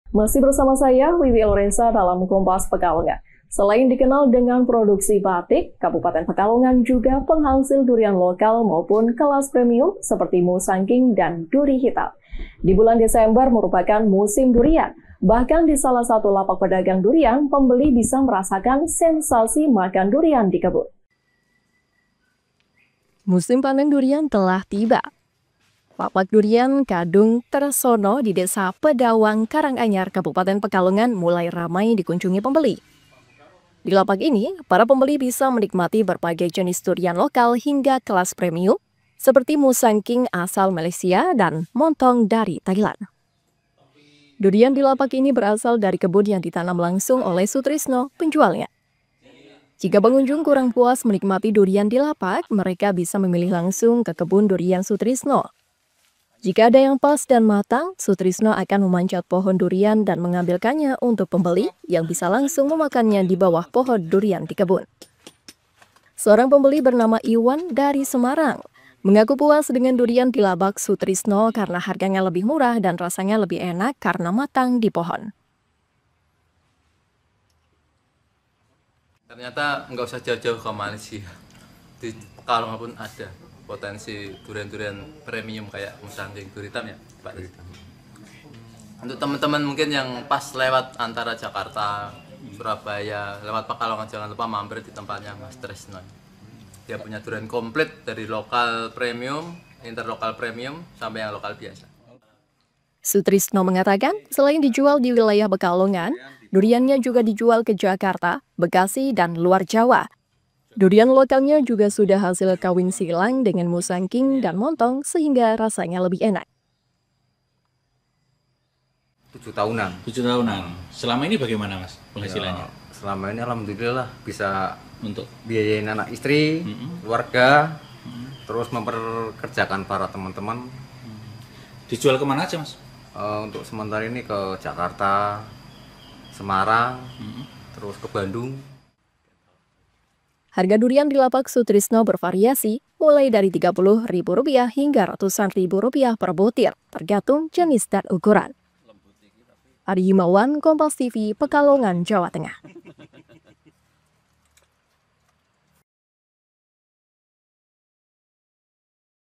Lapak Kadung Tresno: Surganya Pecinta Durian di Pekalongan
PEKALONGAN, KOMPAS.TV - Musim panen durian telah tiba. Lapak Durian Kadung Tresno di Desa Pedawang, Karanganyar, Kabupaten Pekalongan, mulai ramai dikunjungi pembeli. Di lapak ini, para pembeli bisa menikmati berbagai jenis durian lokal sampai dengan kelas premium, seperti Musang King asal Malaysia maupun Monthong Thailand.

Buah durian yang ada merupakan hasil kebun yang ditanam langsung oleh penjualnya, yaitu Sutrisno. Bahkan, jika pengunjung kurang puas menikmati durian di lapak pedagang, mereka bisa memilih langsung ke kebun duriannya. Kalau ada yang pas dan matang, biasanya Sutrisno memanjat pohon durian dan mengambilkannya untuk pembeli. Mereka bisa langsung memakannya di bawah pohon durian langsung di kebun.

Salah seorang pembeli, Iwan, menyebut meski dari Semarang, dirinya puas dengan durian di lapak milik Sutrisno. Pasalnya, harganya jauh lebih murah apalagi jenis Musang King maupun Monthong. Untuk rasanya, bahkan lebih enak karena masak dari pohonnya.

Sementara Sutrisno menyebut, selain dijual di wilayah Pekalongan, durian miliknya juga dijual ke wilayah Jakarta, Bekasi, dan luar Jawa. Durian lokal miliknya bahkan beberapa jenis sudah hasil kawin silang dengan Musang King maupun Monthong sehingga rasanya lebih enak.

Untuk harga, pembeli bisa menyesuaikan dengan isi kantong. Sutrisno biasanya menjual buah durian dari harga Rp30 ribu sampai dengan ratusan ribu rupiah per butir, tergantung jenis dan ukuran.